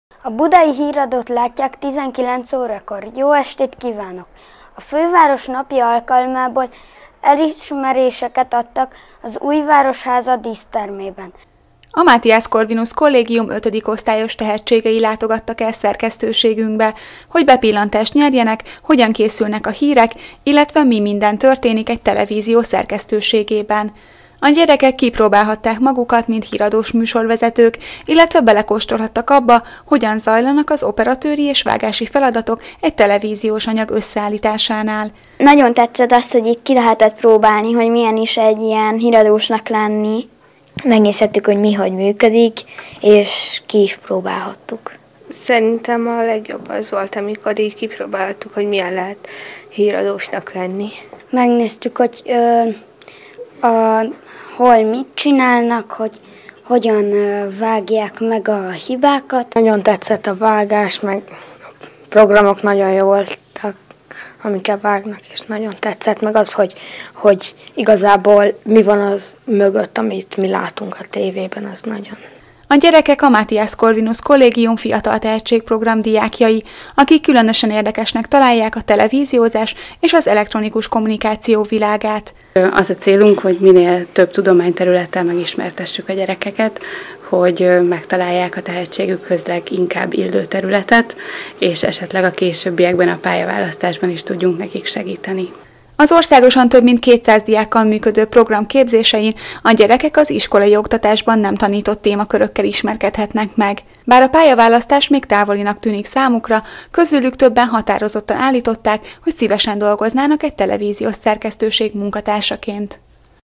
MCC FIT Program - Budapesti diákok a Hegyvidék Tv stúdiójában
Az MCC Fiatal Tehetség Programjának fővárosi diákjai a Hegyvidék Tv stúdiójába látogattak. A velük készült rövid interjúk mellett Kovács Cecília régióvezető is nyilatkozik.